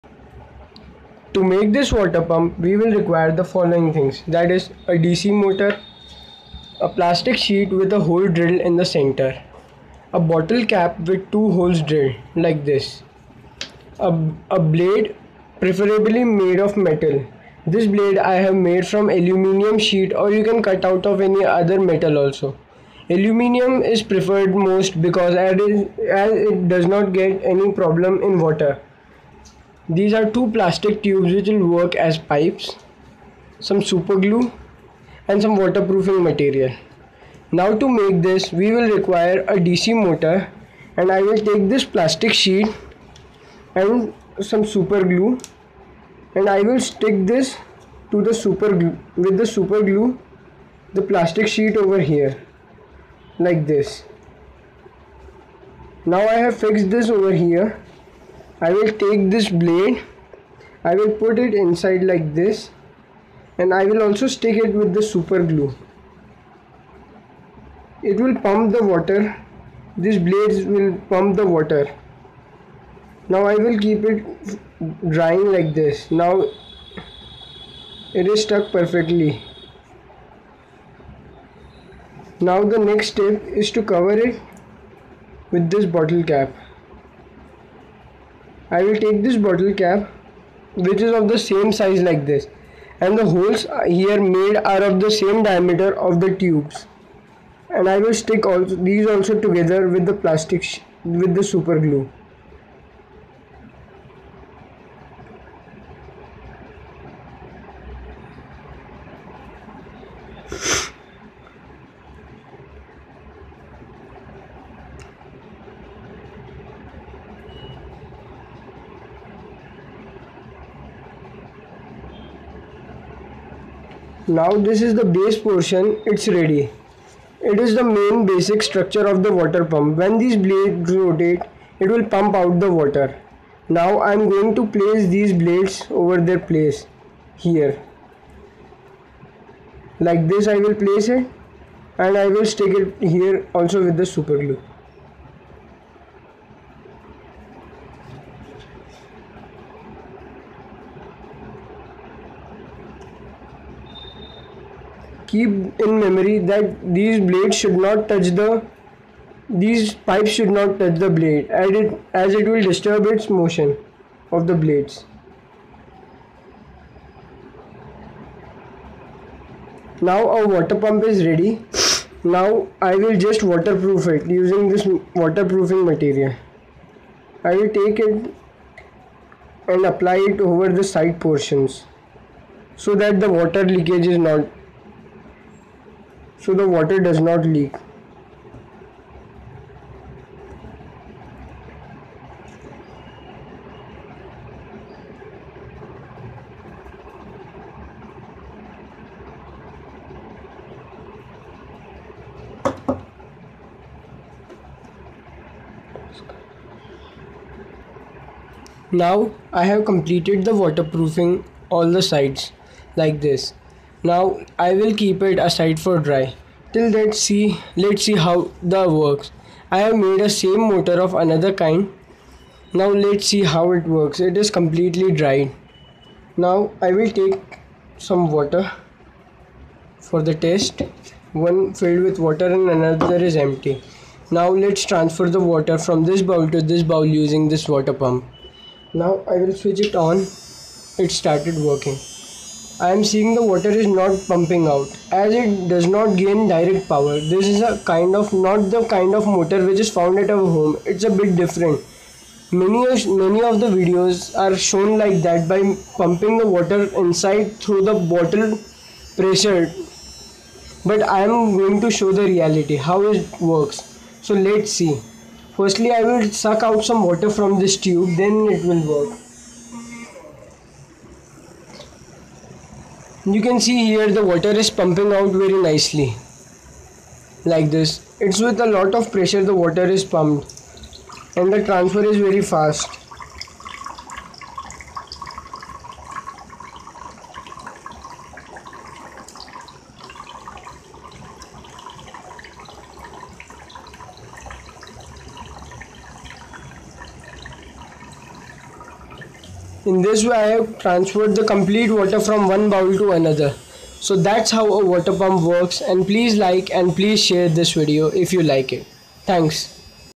Mini water pump
This video enables you to make a easy water pump which actually works at nominal cost.